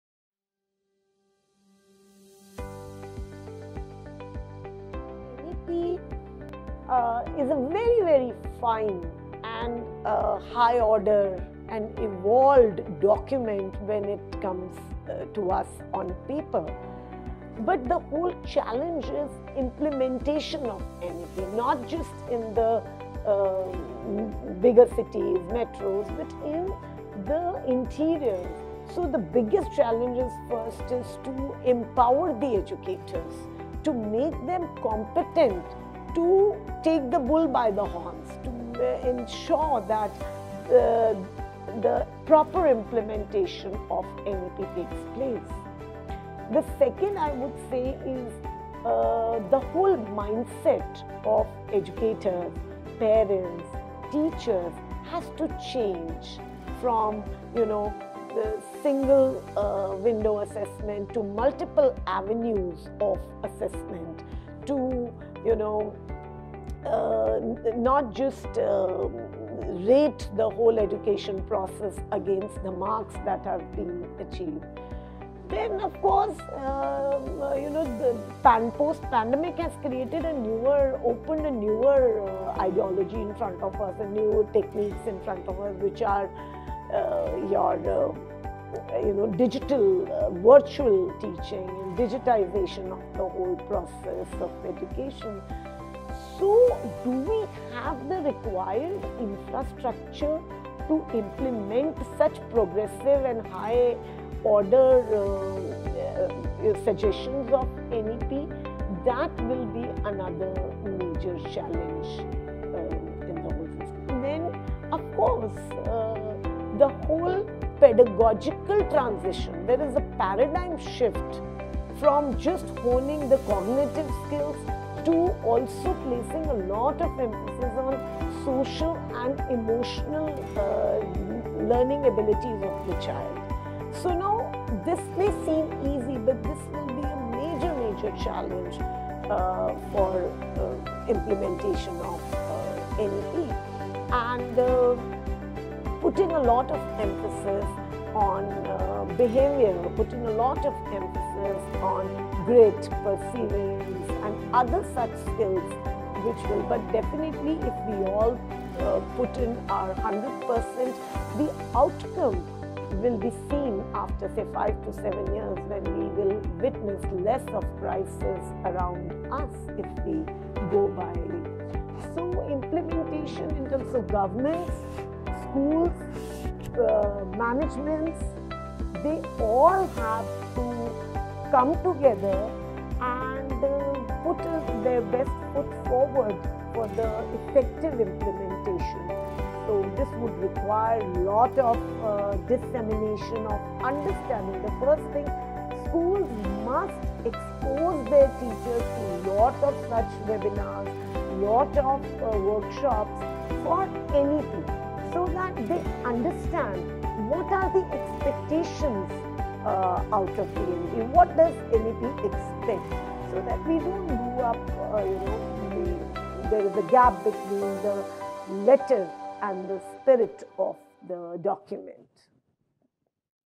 What are the challenges in the implementation of NEP - 2020?
Change in the mindset of all the stakeholders, empowerment of educators, availability of infrastructure, assessment of creative and unorthodox systems of learning, and many more.